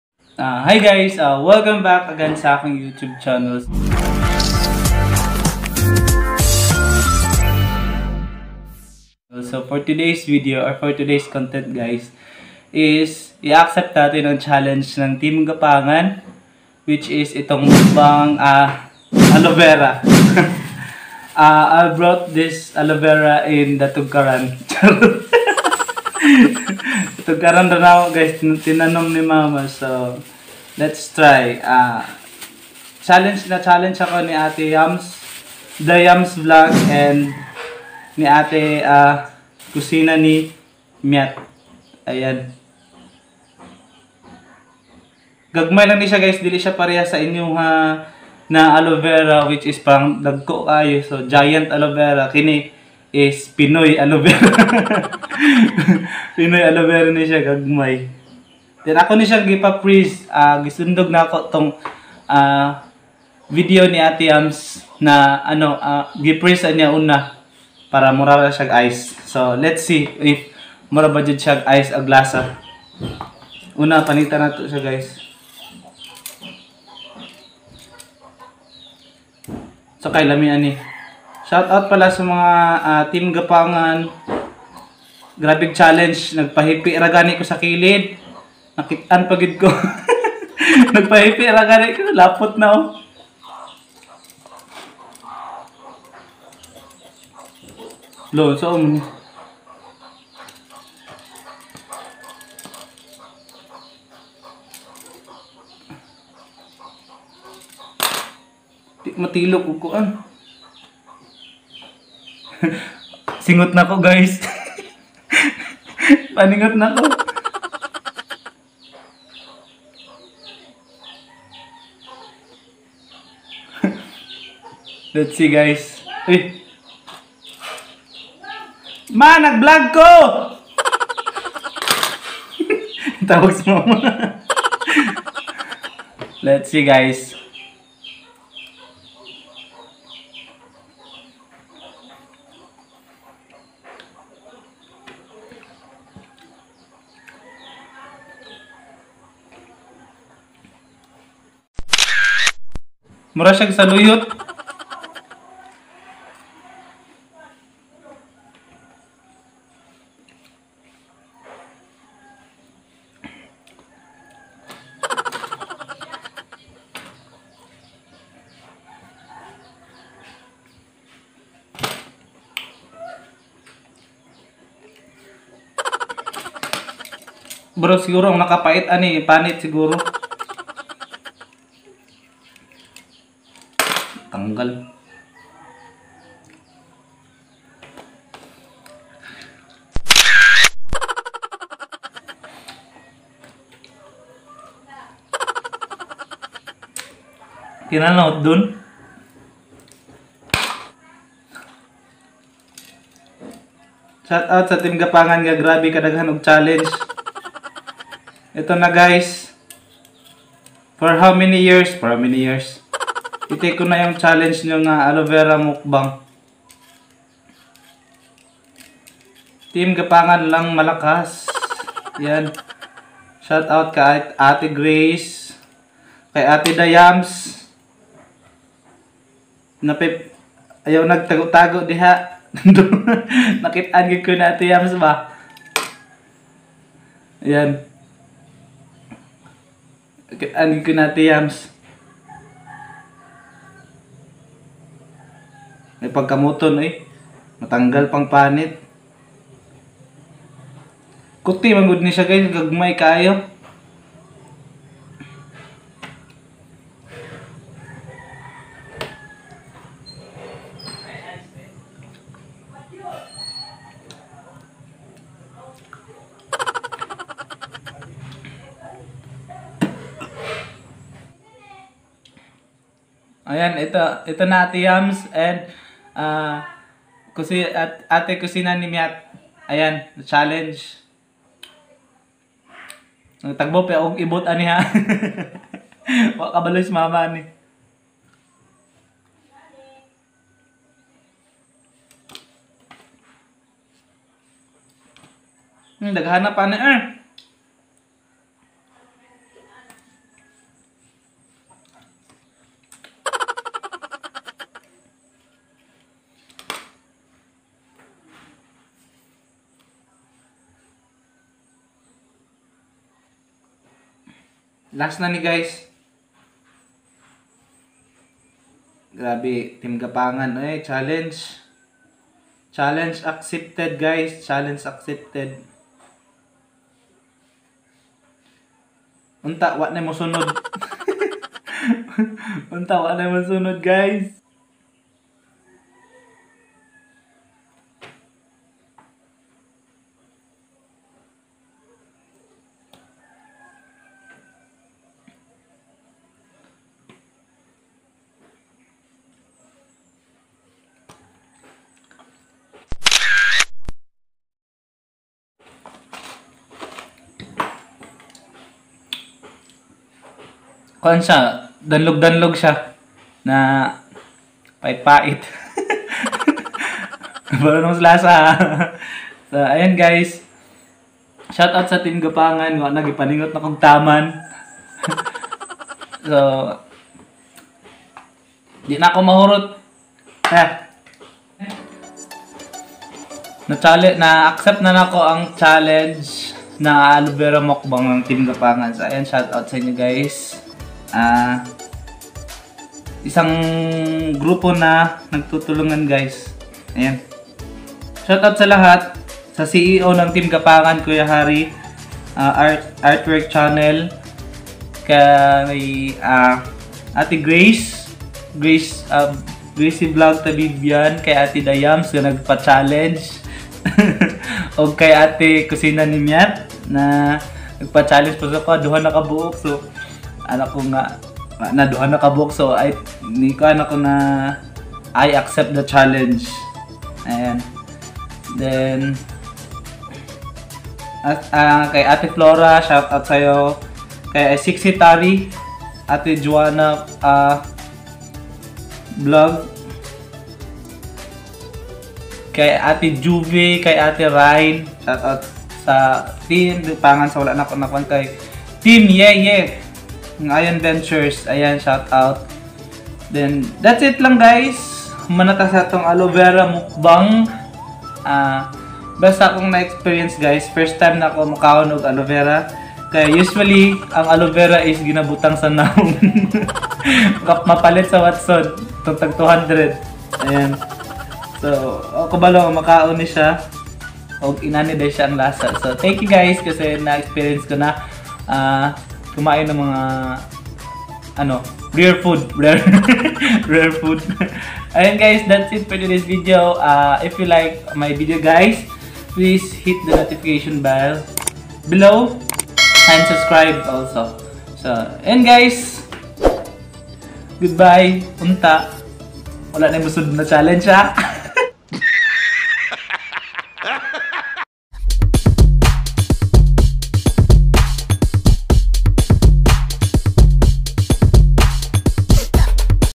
Aloevera mukbang Challenge accepted!!
Aloevera mukbang challenge ng team gapangan
Nagtaka cguro kayo kasi maliit aloevera ko may lahi po yang pinoy HAHAHAHHAHA..

aloe vera mukbang challenge accepted